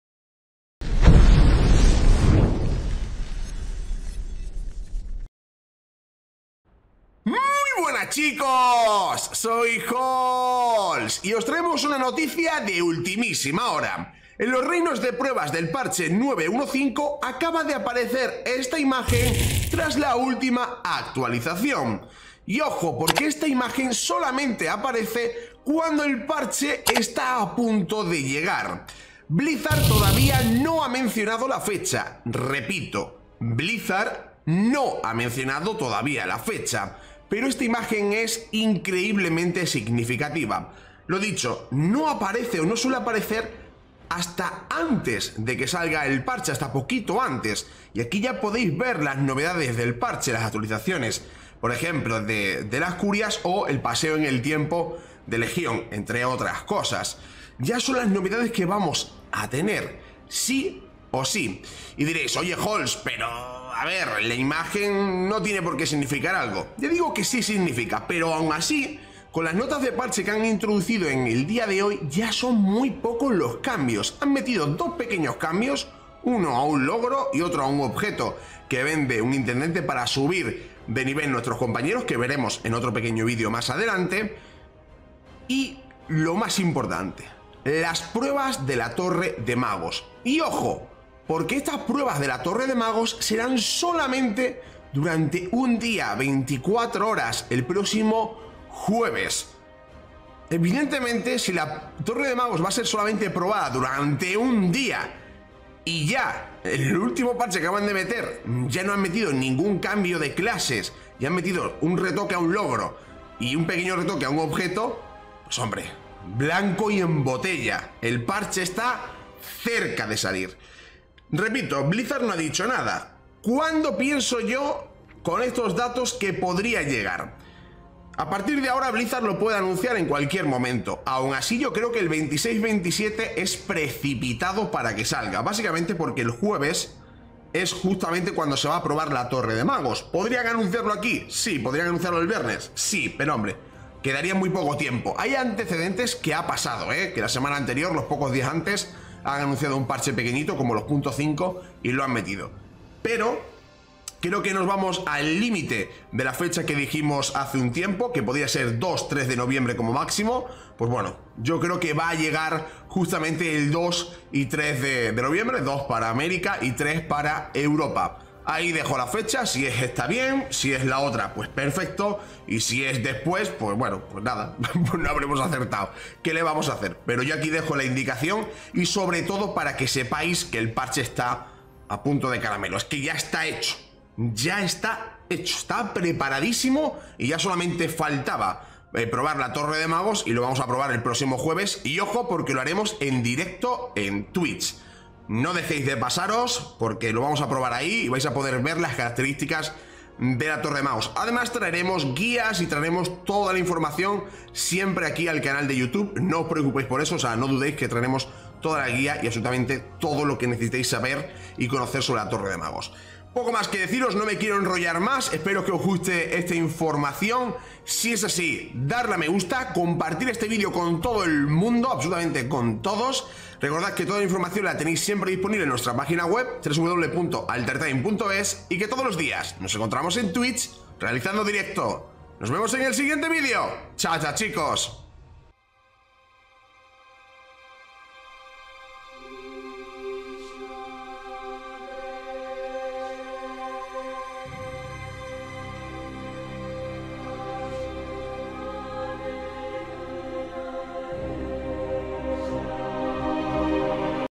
PARCHE 9.1.5 MUY CERCA | Pruebas TORRE DE MAGOS el próximo jueves
Blizzard ha lanzado esta madrugada una actualización para el Reino de Pruebas y todo parece indicar que el Parche 9.1.5 llegará muy pronto. En el vídeo analizamos fechas y la noticias de la prueba de la Torre de Magos.

ALTERTIME, FANSITE DE BLIZZARD